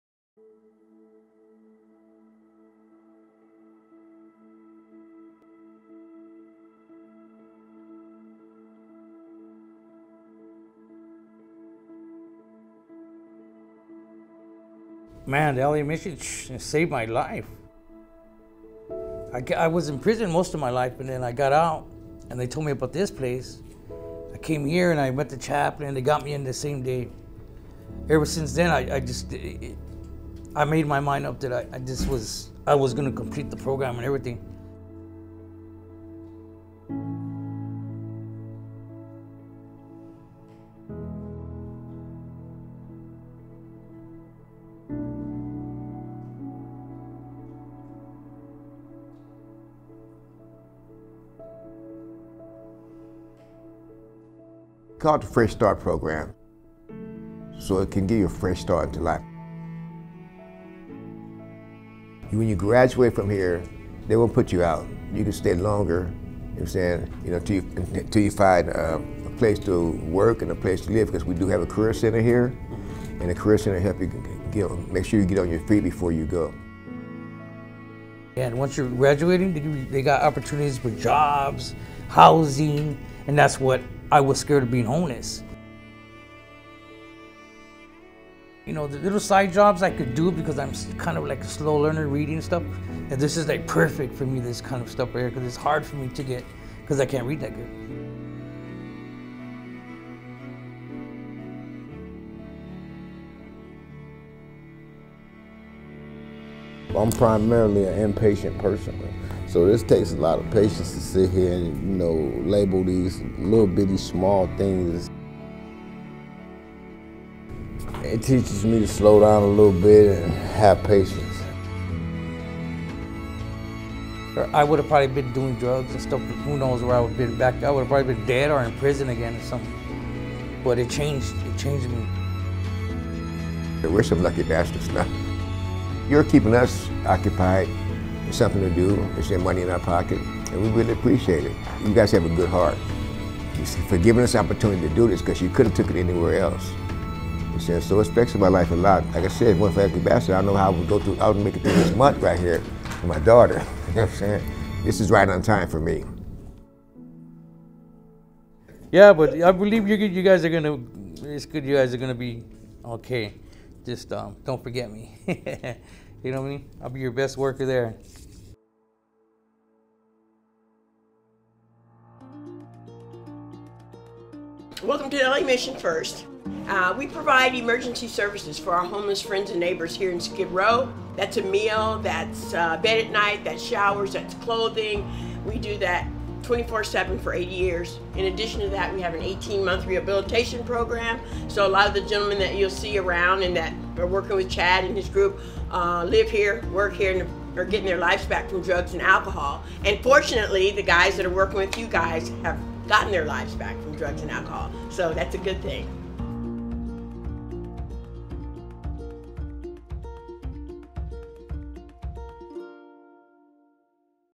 Lucky Bastard Co Hires Los Angeles Mission Recovered Homeless
The Los Angeles Mission is a homeless recovery center is DTLA's skin row. Since 1932 it has provided a safe place to eat, sleep and bathe for 400 people in need. After an 18 month rehabilitation program, graduates begin to look for job opportunities to get their life back on track. We have hired a group a guys who have labeled over 100,000 lip balms and counting. Here is what Lucky Bastard Co means to them.